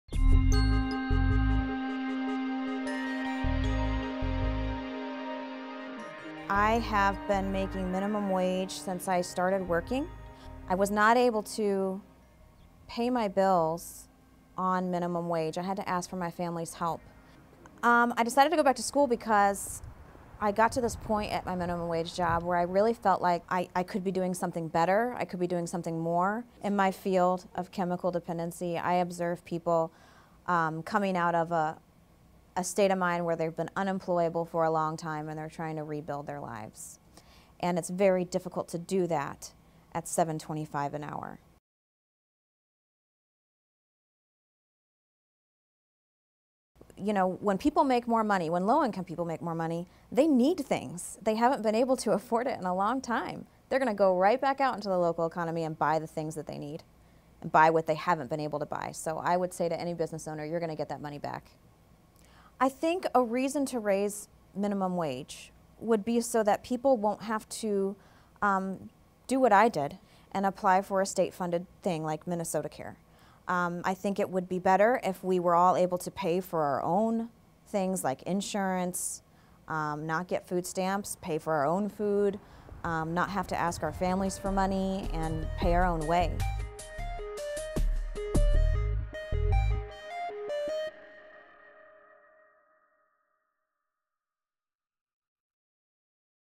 MN2020: Breaking the Poverty Cycle
Eva Pickeral knows first-hand raising minimum wage would flood the local economy with money that's been shorthanded for too long. She is a recent transplant in search of a second chance. She worked full time paycheck to paycheck in minimum wage jobs, and still relied on MinnesotaCare to help cover health care.
Now she's going back to school at MCTC, in part to get certified as a licensed alcohol and drug counselor to help empower other working people in recovery to rejoin economy, like she once did herself.